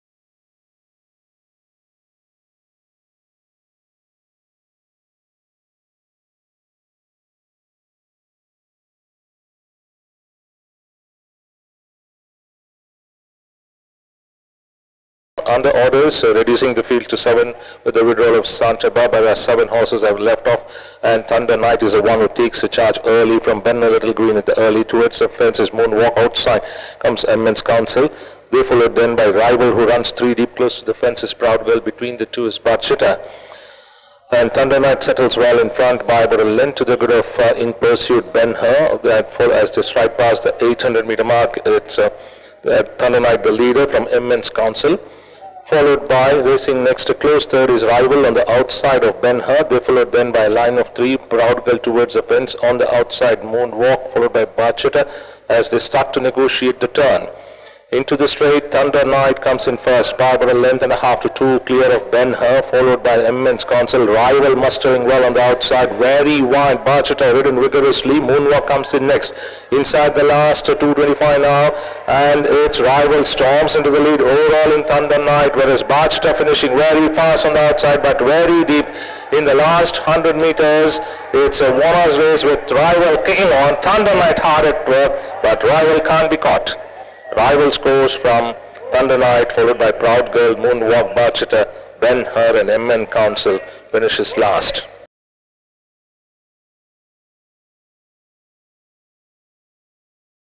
Rival with Santosh Raj N R up wins The Silver Fortress Plate Div-1 2023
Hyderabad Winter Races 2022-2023
Rival trained by K S V Prasad Raju with Santosh Raj N R up wins The Silver Fortress Plate Div-1 2023 at The Hyderabad  Race Club